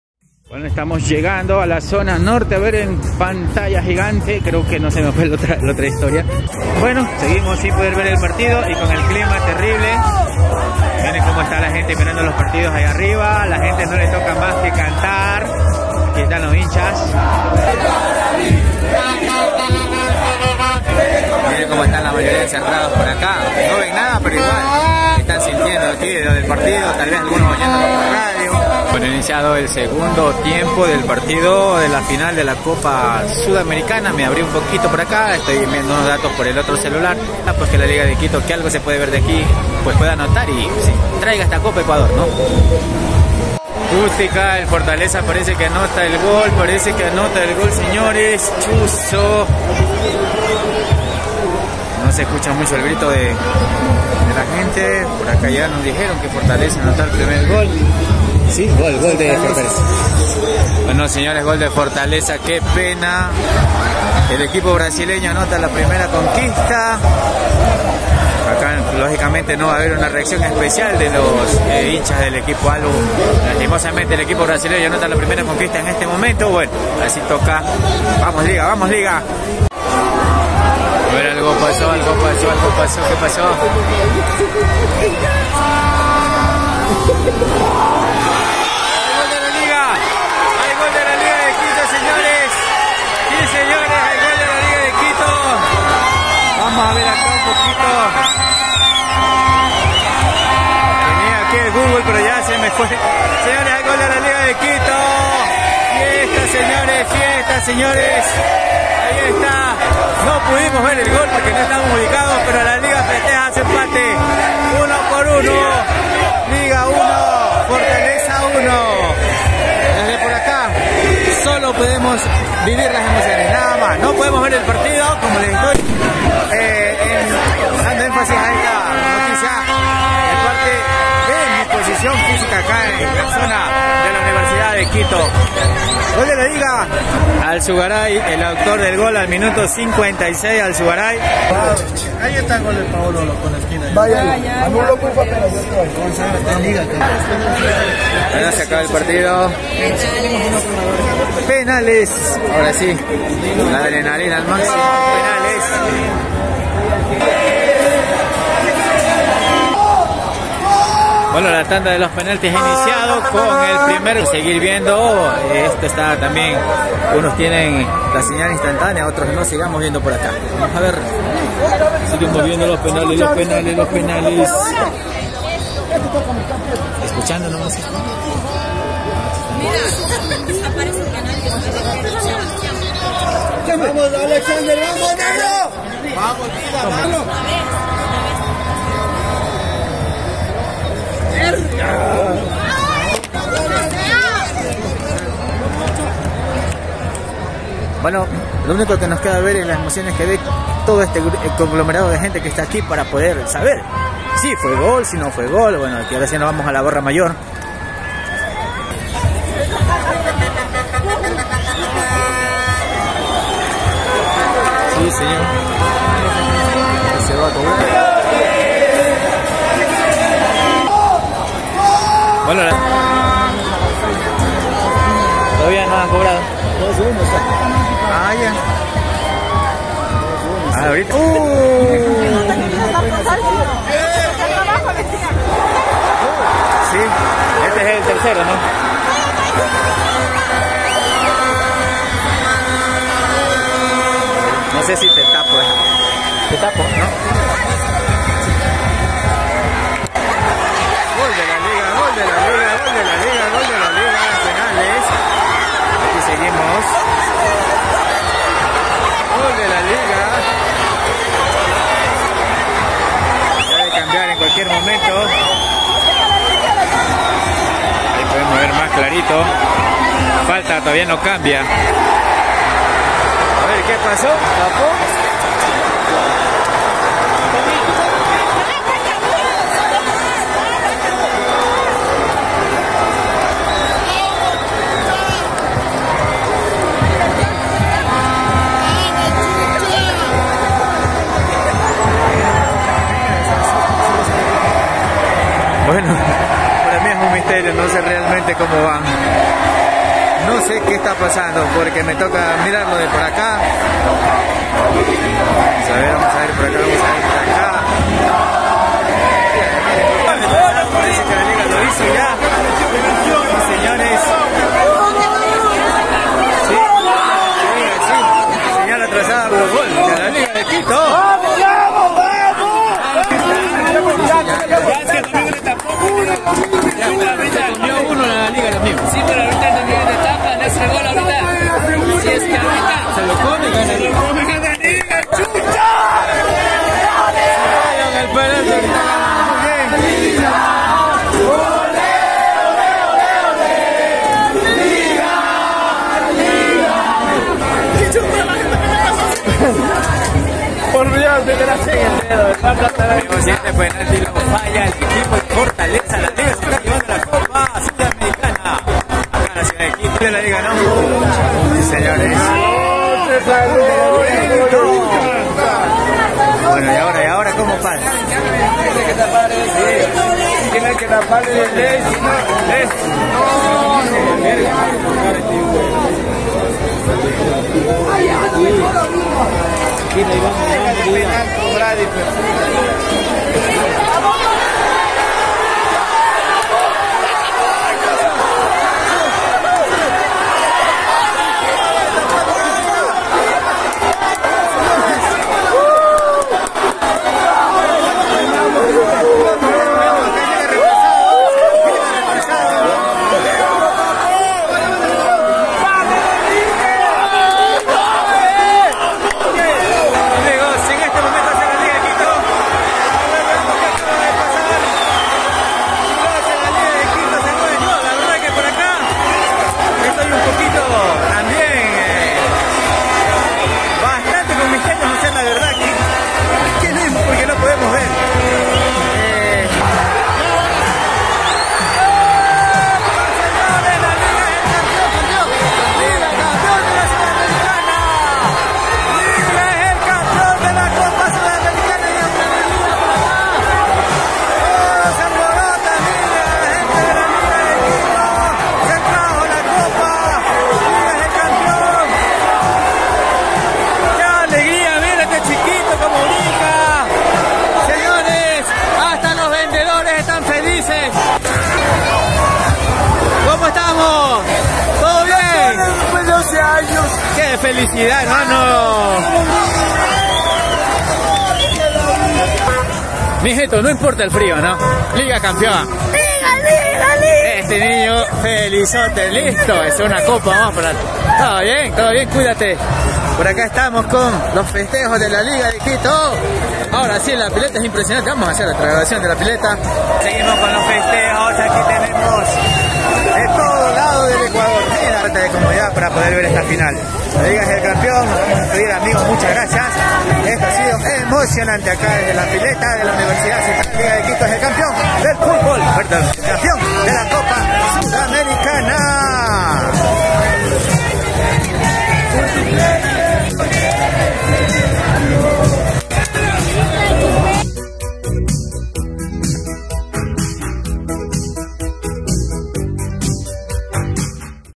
Final de la Copa Sudamericana 2023 : Fortaleza y Liga de Quito - REACCIONES EN QUITO
LIGA DE QUITO CAMPEON COPA SUDAMERICANA!! EN PENALES!
FESTEJOS EN LA CAPITAL, PILETA DE U. CENTRAL